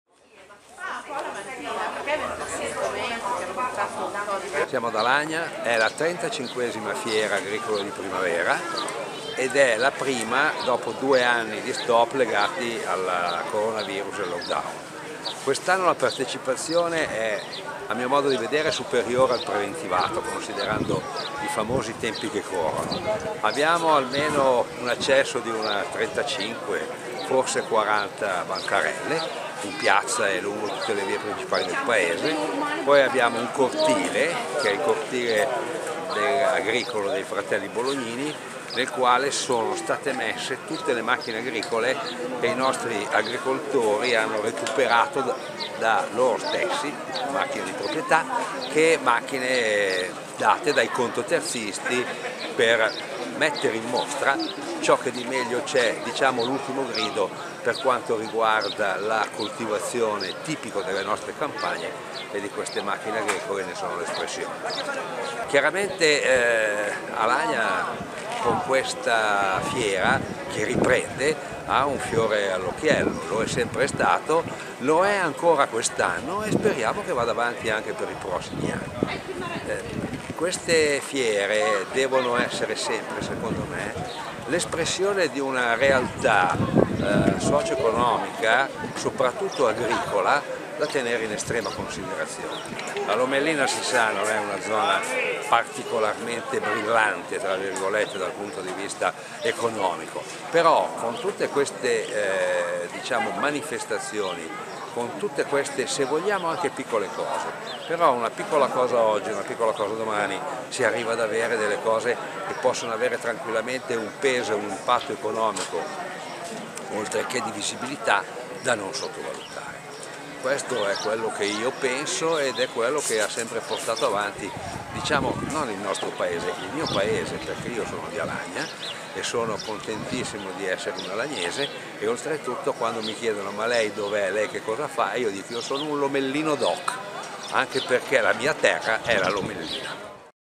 Alagna, torna la fiera agricola
Torna dopo due anni la fiera agricola di Alagna: 40 banchetti e l'esposizione dei mezzi agricoli. Parla il sindaco Riccardo Ferrari. Video Morandi